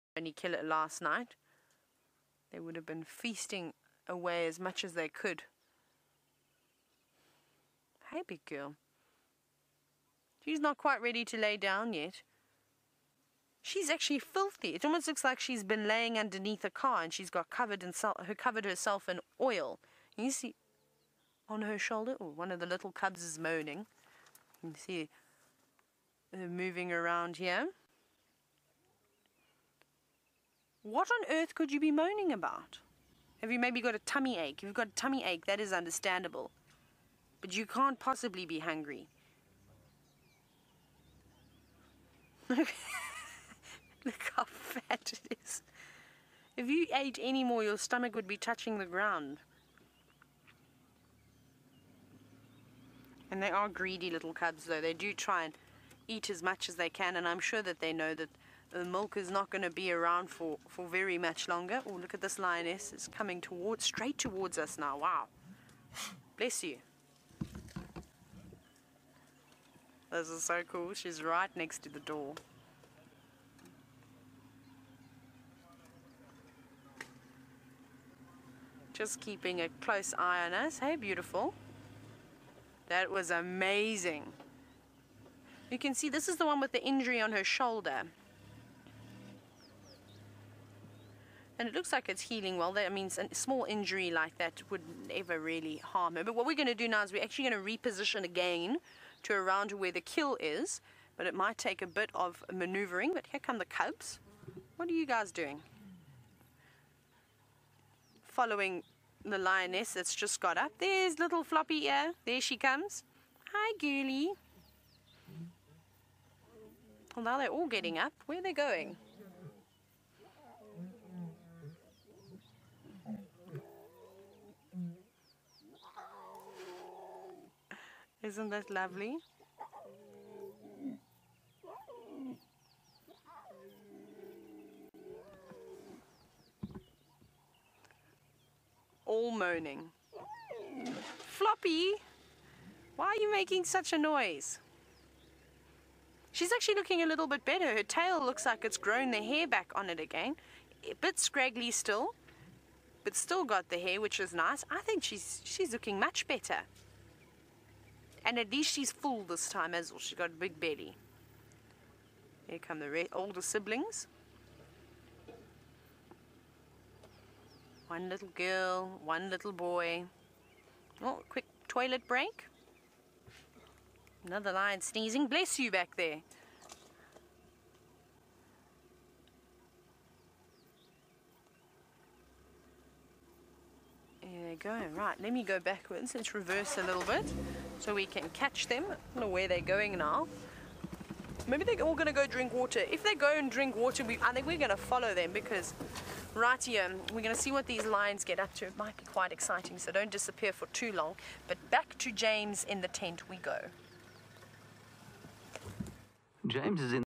Jan 7, 2017- Nkuhuma Cubs have FAT bellies and Floppy Ear is getting better
(sorry if video is blocky- FC had transmission issues first hour)